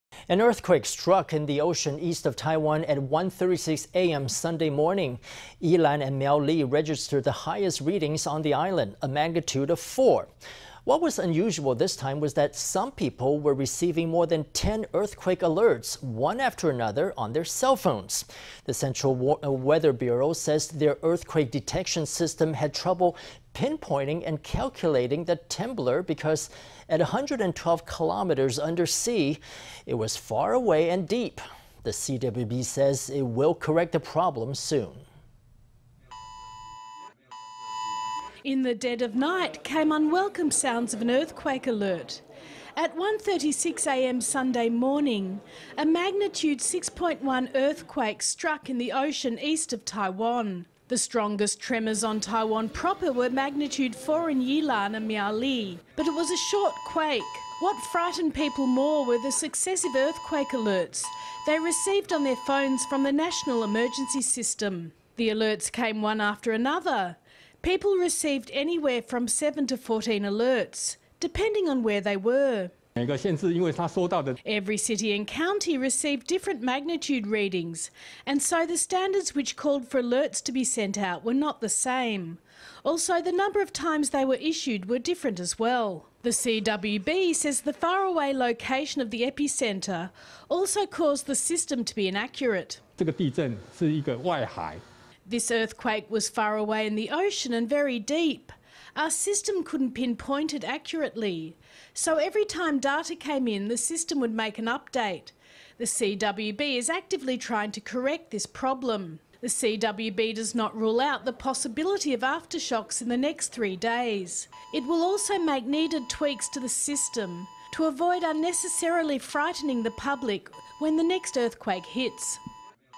Depth of earthquake led to difficulty in pinpointing and calculating it: CWB
An earthquake struck in the ocean east of Taiwan at 1:36 a.m. Sunday morning. Yilan and Miaoli registered the highest readings on the island: a magnitude of 4. What was unusual this time was that some people were receiving more than 10 earthquake alerts, one after another, on their cell phones. The Central Weather Bureau says their earthquake detection system had trouble pinpointing and calculating the temblor because, at 112 kilometers undersea, it was faraway and deep. The CWB says it will correct the problem soon.

In the dead of night came unwelcome sounds of an earthquake alert. At 1:36 a.m. Sunday morning, a magnitude 6.1 earthquake struck in the ocean east of Taiwan. The strongest tremors on Taiwan proper were magnitude 4 in Yilan and Miaoli. But it was a short quake. What frightened people more were the successive earthquake alerts they received on their phones from the national emergency system.

The alerts came one after another. People received anywhere from seven to 14 alerts, depending on where they were.

Wu Chien-wen
Seismological Center, CWB
Every city and county received different magnitude readings and so the standards which called for alerts to be sent out were not the same. Also, the number of times they were issued were different as well.

The CWB says the faraway location of the epicenter also caused the system to be inaccurate.

Wu Chien-wen
Seismological Center, CWB
This earthquake was faraway in the ocean and very deep. Our system couldn''t pinpoint it accurately. So every time data came in, the system would make an update. The CWB is actively trying to correct this problem.

The CWB does not rule out the possibility of aftershocks in the next three days. It will also make needed tweaks to the system to avoid unnecessarily frightening the public when the next earthquake hits.